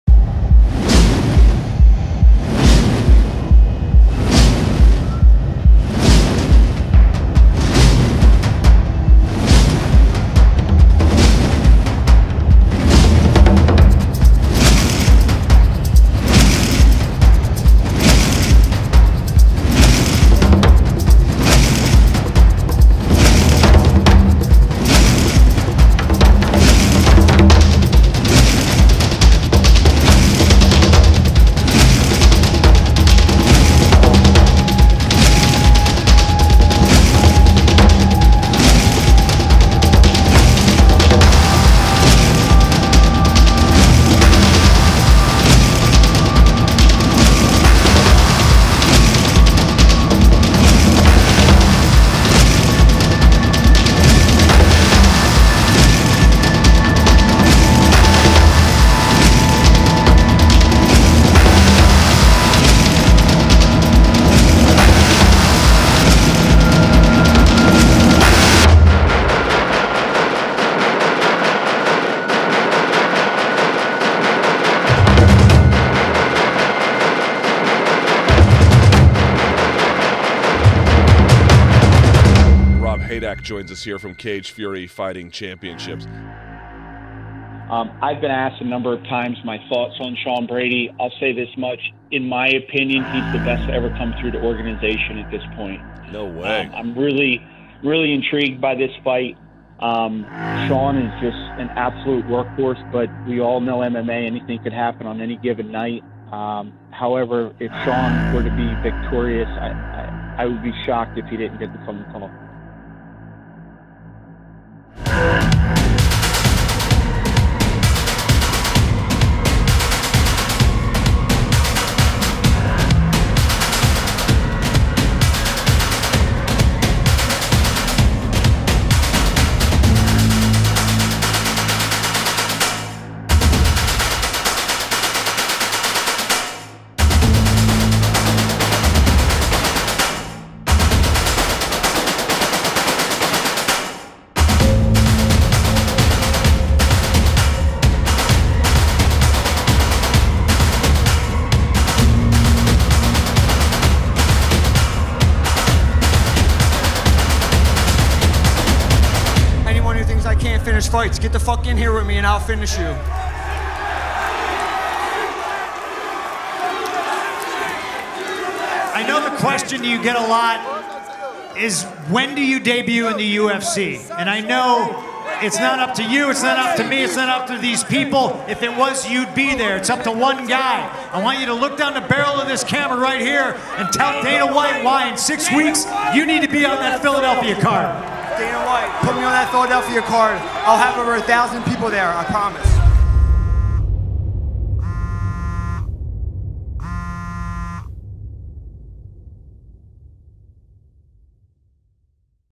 Sean Brady CFFC MMA career highlights (Pre-UFC)
Another champion answers the call after a successful CFFC career and heads to the big show.